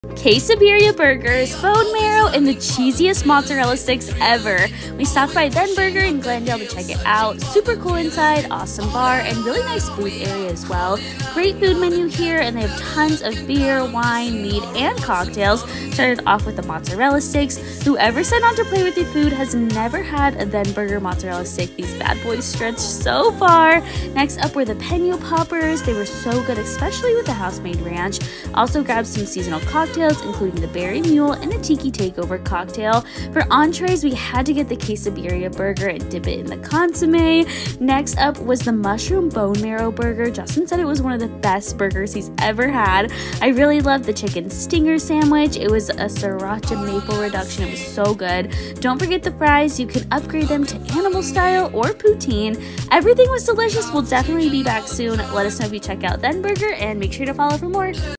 Quesabirra Burgers & bone Marrow Burgers! New burger spot in Phoenix!
If you haven’t been to @thenburger yet, GO! Some of the best burgers, drinks and mozzarella sticks EVER! This is such a good spot! Definitely check it out if you are on the west side and make the drive if you aren’t - worth it!!

📍Then Burger
9220 W Glendale Ave
Unit 100